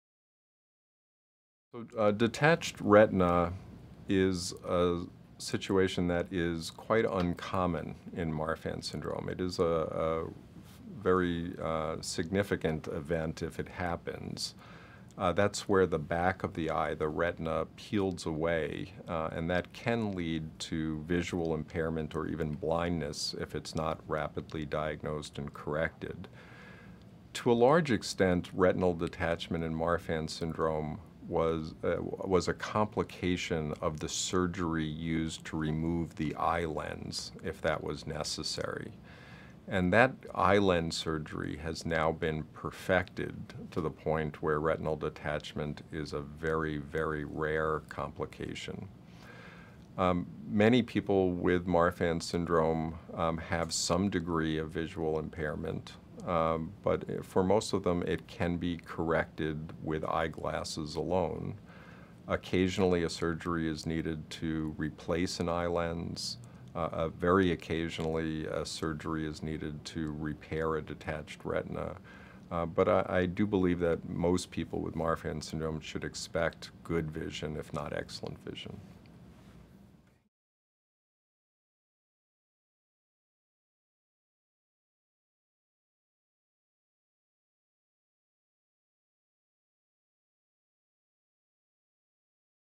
What are the issues with eyes in Marfan syndrome?
Hal C. Dietz, MD, is Director of the William S. Smilow Center for Marfan Research at Johns Hopkins Hospital, Victor A. McKusick Professor of Medicine and Genetics at Johns Hopkins Hospital, and Investigator in the Howard Hughes Medical Institute. A current member and former chair of the Foundation's Professional Advisory Board, Dr. Dietz's specialties include pediatrics, cardiology, and genetics.